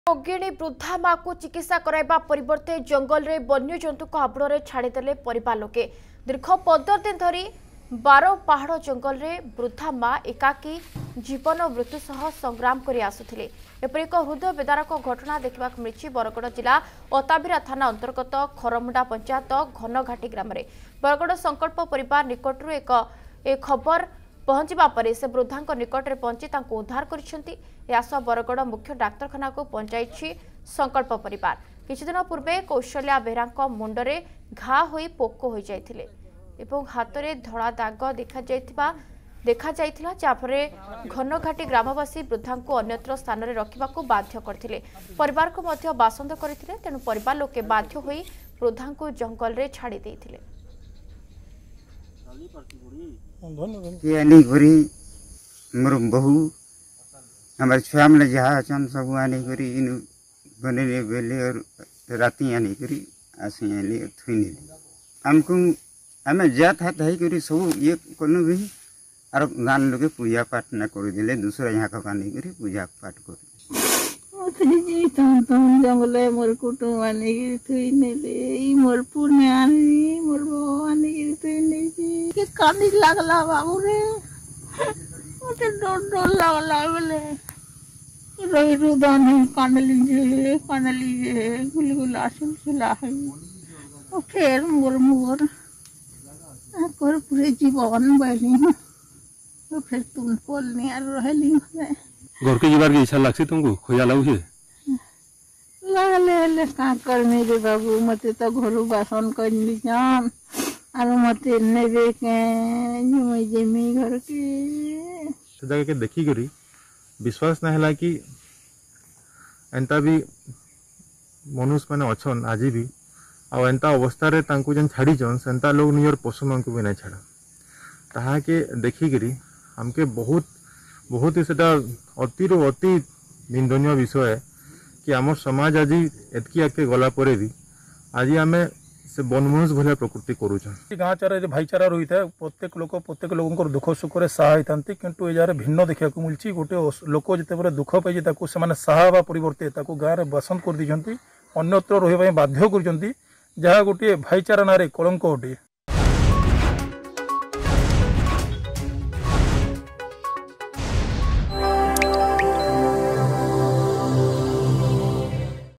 ବୃଦ୍ଧା ମାଙ୍କୁ ଚିକିତ୍ସା କରାଇବା ପରିବର୍ତ୍ତେ ଜଙ୍ଗଲରେ ଛାଡ଼ିଦେଲେ ପରିବାର
ରୋଗିଣୀ ବୃଦ୍ଧା ମାଙ୍କୁ ଚିକିତ୍ସା କରାଇବା ପରିବର୍ତ୍ତେ ଜଙ୍ଗଲରେ ବନ୍ୟ ଜନ୍ତୁଙ୍କ ହାବୁଡ଼ରେ ଛାଡ଼ିଦେଲେ ପରିବାର ଲୋକେ। ଦୀର୍ଘ ୧୫ ଦିନ ଧରି ବାରପାହାଡ ଜଙ୍ଗଲରେ ବୃଦ୍ଧା ମା ଏକାକୀ ଜୀବନ ଓ ମୃତ୍ୟୁ ସହ ସଂଗ୍ରାମ କରିଆସୁଥିଲେ।